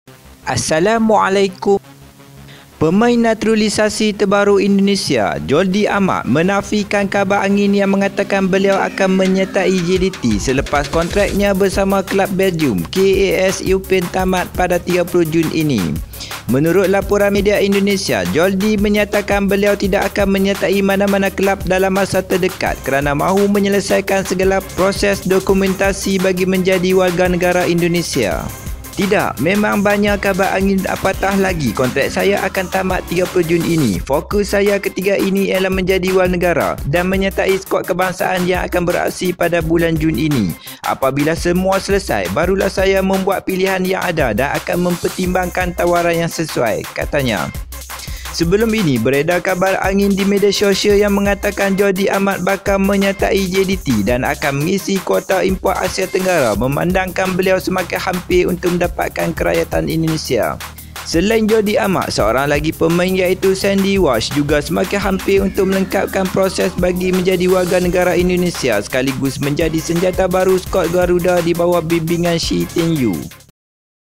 Sah - Ini Pengakuan Sendiri Jordi Amat Status Perpindahan ke JDT - Liga Malaysia 2022
Desas Desus perpindahan Jordi Amat ke Johor Darul Takzim / JDT akhirnya terjawab

PENAFIAN!!
"Penggunaan Gambar Dan Visual hanyalah Naratif Penceritaan Video"
Ia Milik mereka yang berkenaan dan saya berikan kredit kepada mereka.